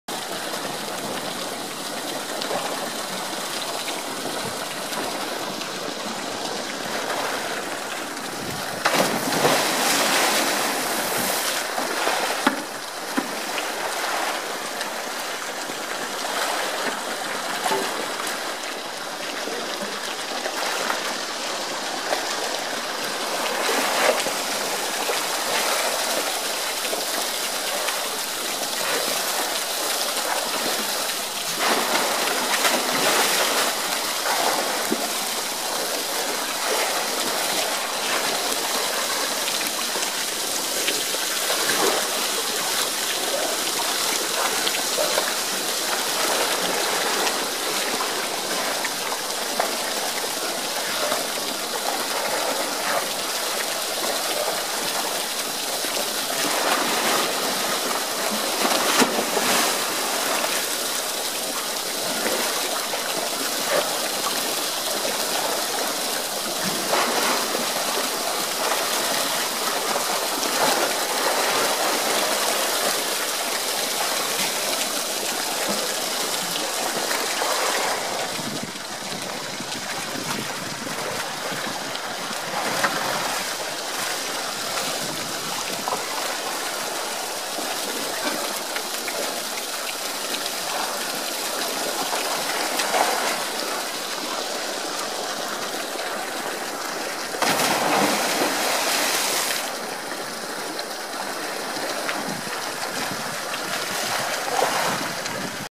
遊ぶの飽きた？
なぜかこの日はあまり遊びません。
どうしたのかな。前日に遊びすぎたのかな。